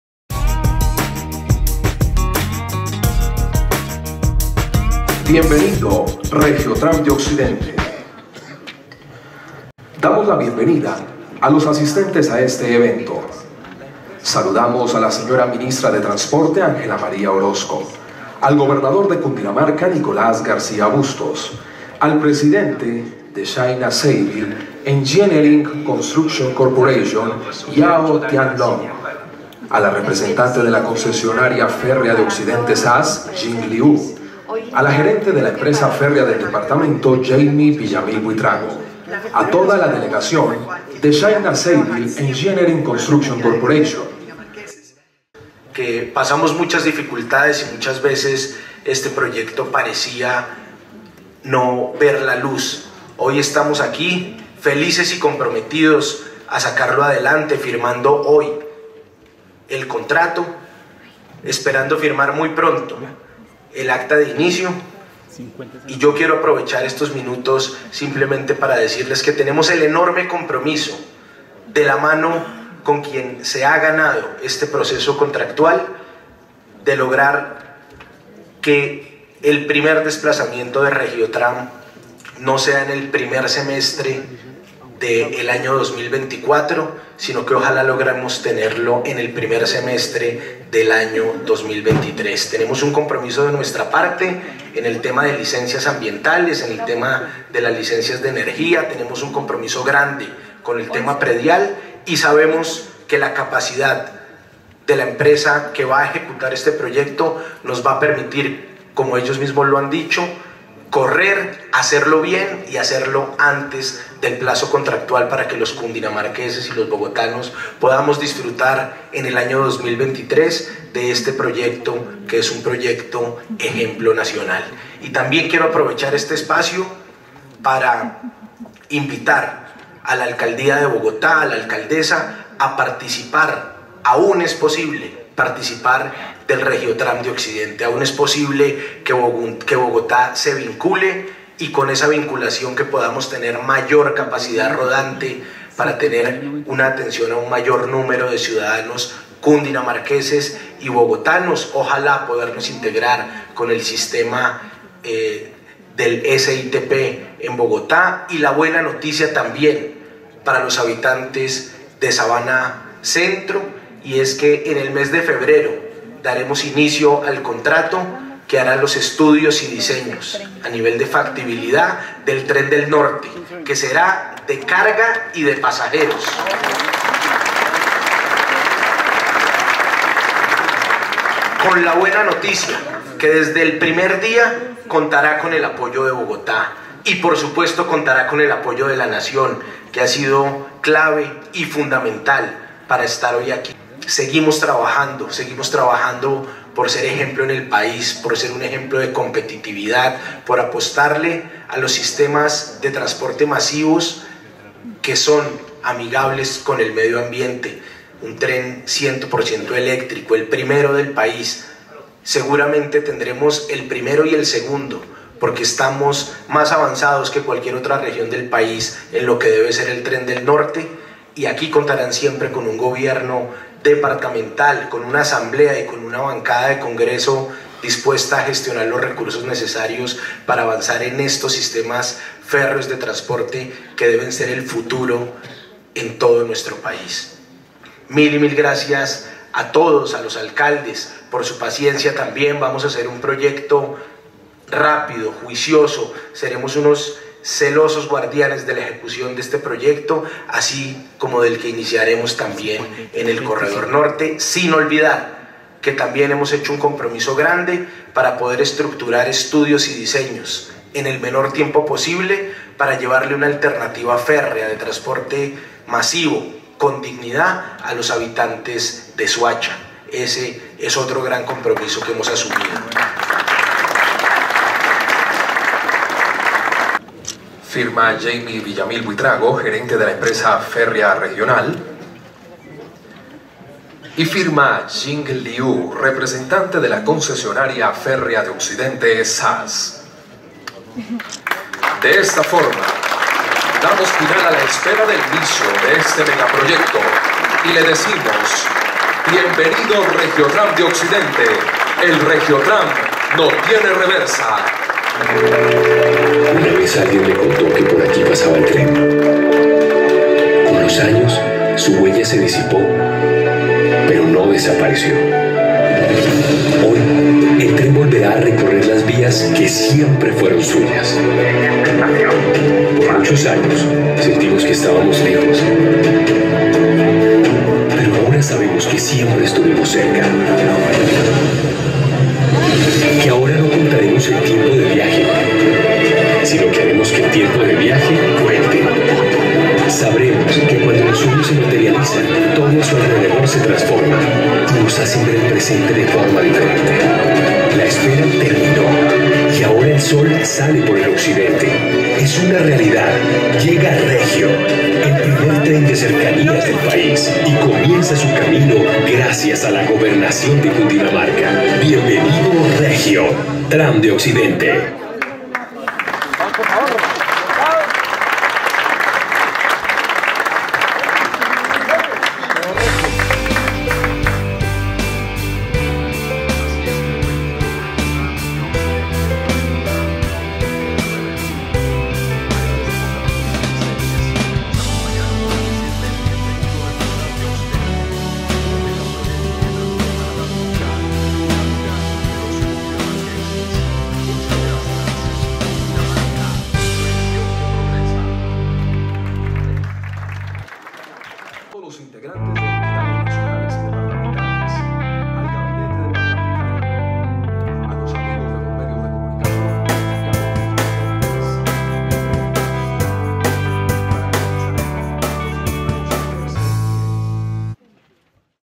Distrito Capital comprometido con RegioTram
La doctora Claudia Lopez, alcaldesa de Bogotá y gobernador de Cundinamarca Nicolas Garcia Bustos planean sus acuerdos para avanzar en el desarrollo de Bogotá Región.